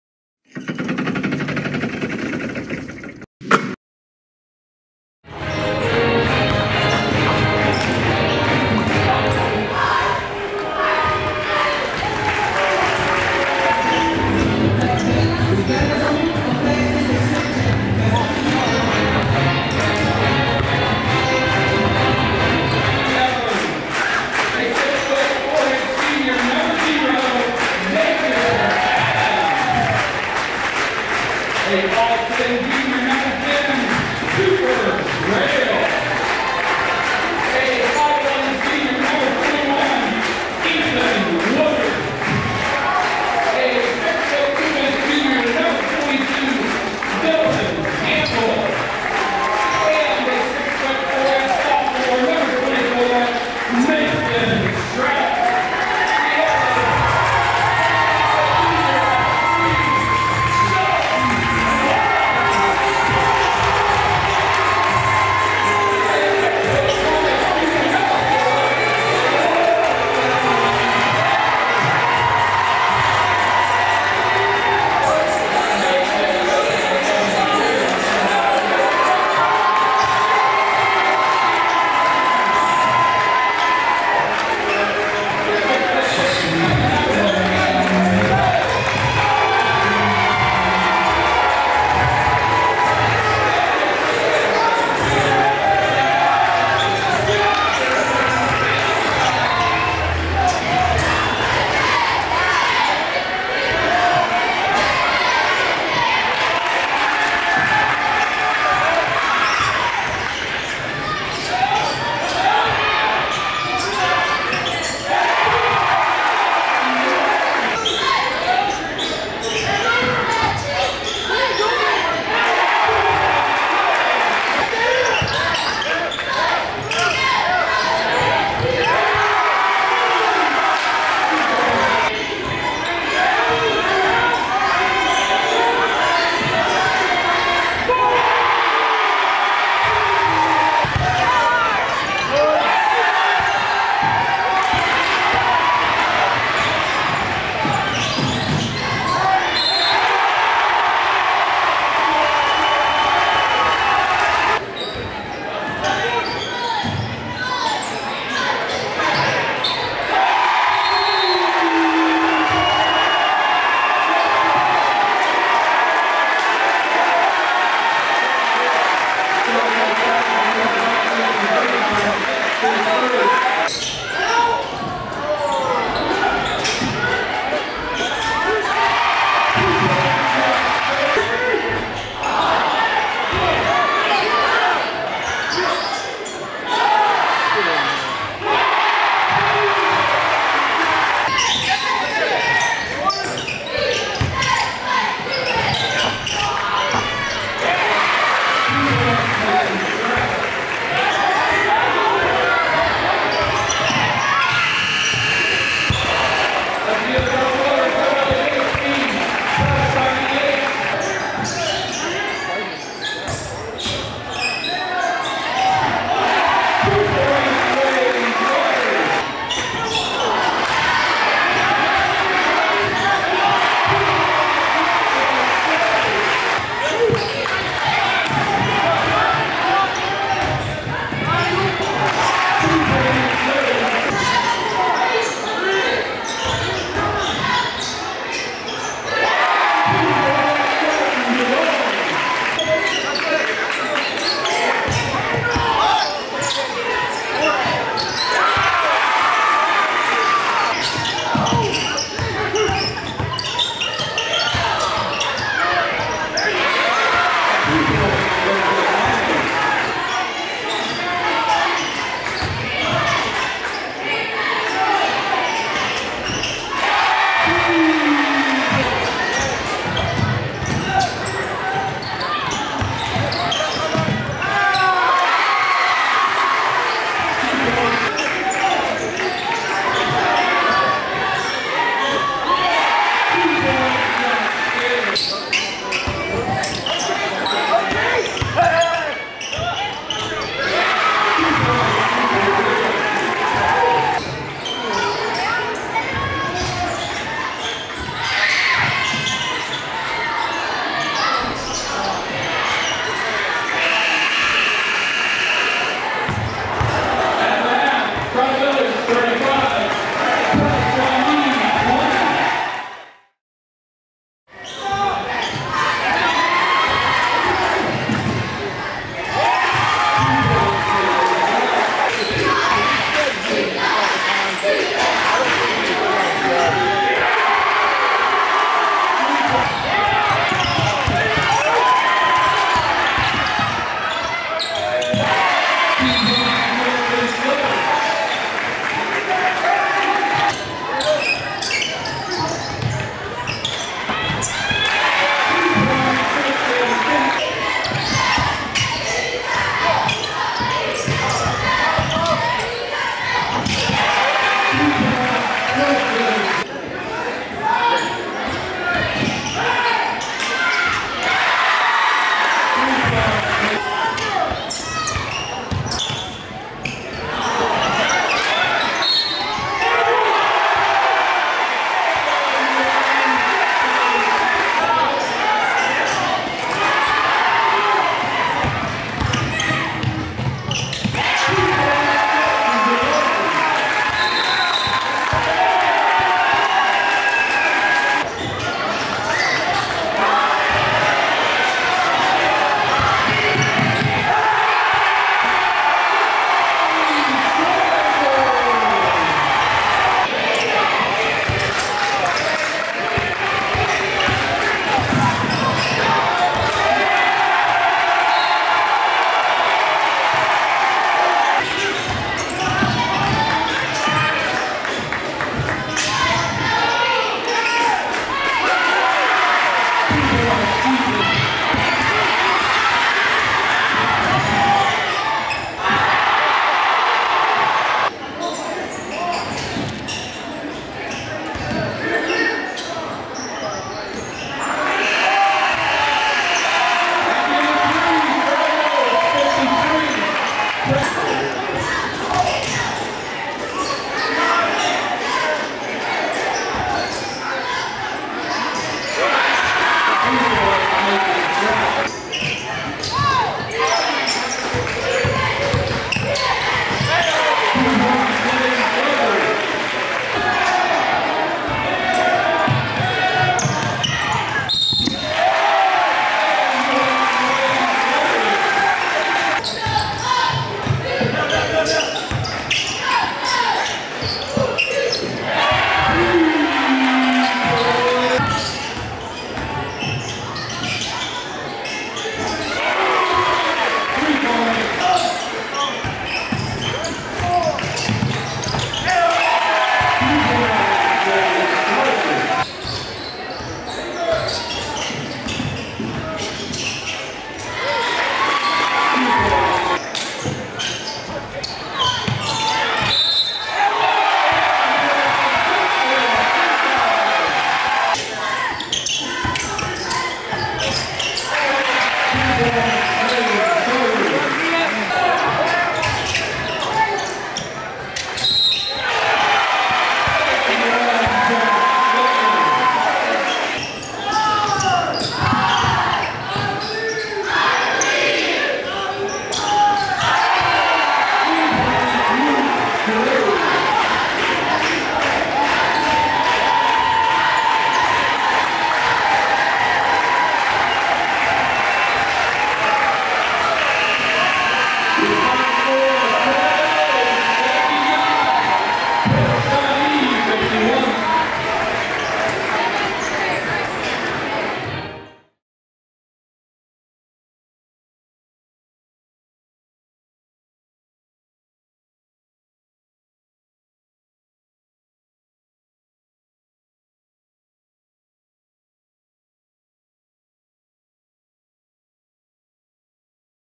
Tri-Village secures the WOAC title after a dominant win over Preble Shawnee! (Full Game Highlights)
Tri-Village secured the WOAC title outright last night after their dominant 79-51 win over Preble Shawnee. The Patriots were led by Layne Sarver who had a game-high 28 points. Dalton Delong, Josh Scantland, Justin Finkbine, Wilson Suggs, and Jason Hale all contributed big minutes as well as they jumped out to an early lead and didn’t look back. Mason Shrout led Preble Shawnee with 22 points but it wasn’t enough as the Arrows fall to 13-3 on the year.